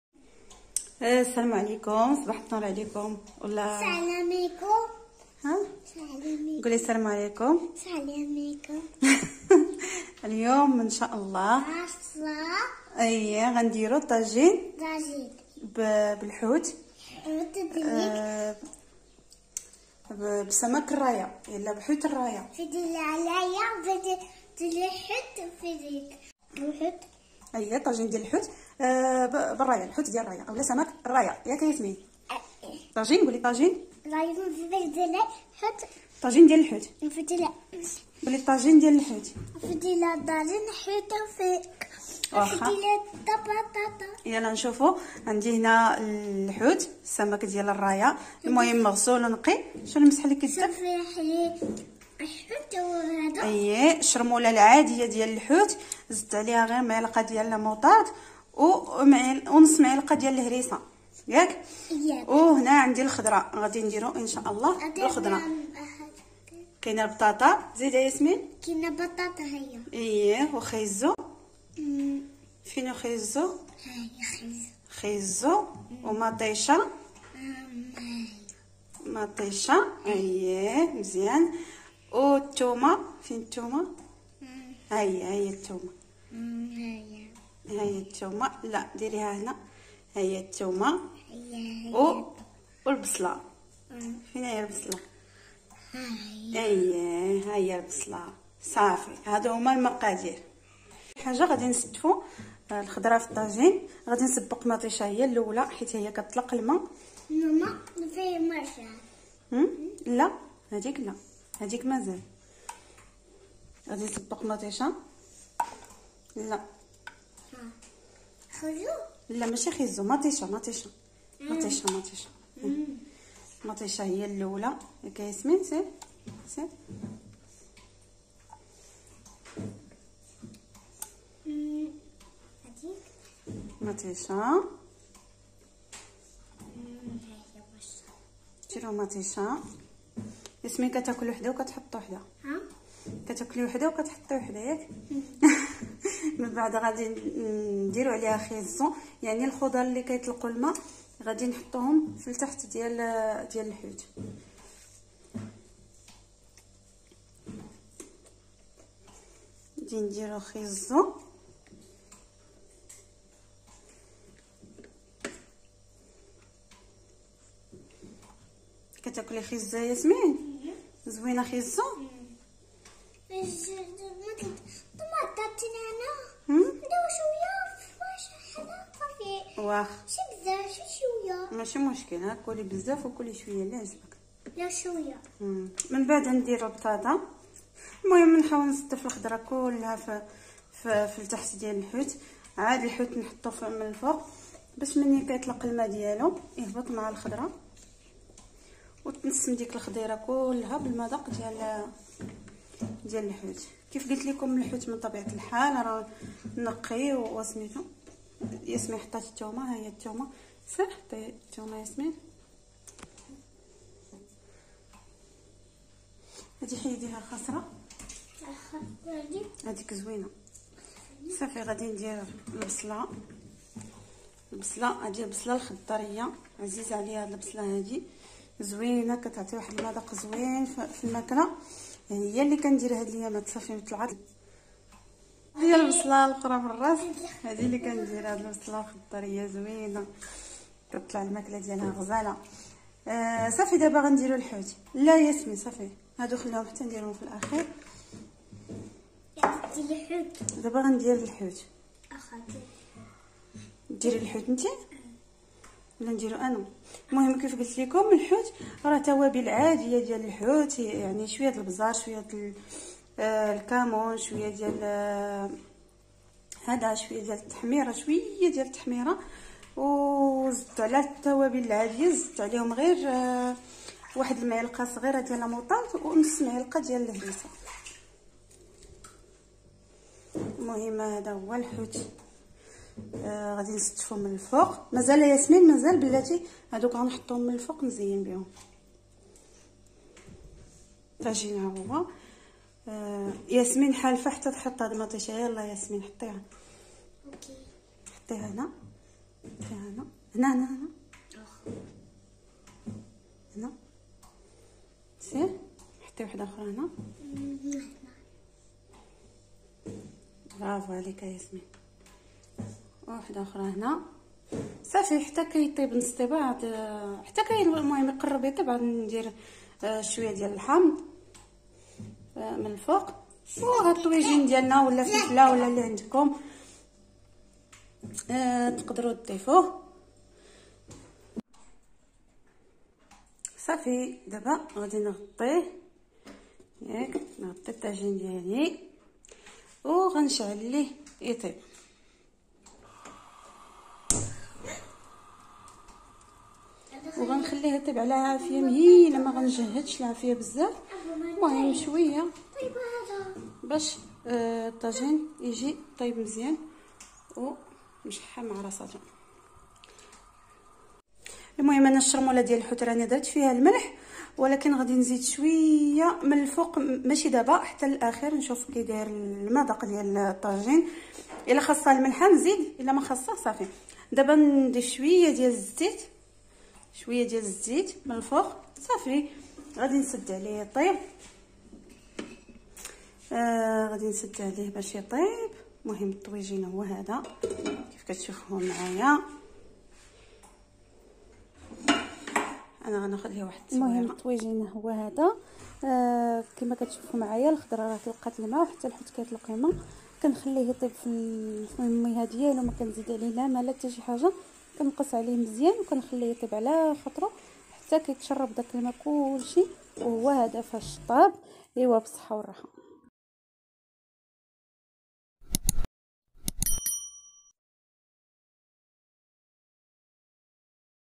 طاجين_الحوت شهوة منو 🐟Tajine de poisson
طريقة تحضير طاجين سمك الراية بالخضر صحي ولذيذ بأبسط طريقة وبمكونات متوفرة في اي بيت وفرمشة العين كيوجد مفيد جدا خصوصا فهاذ البر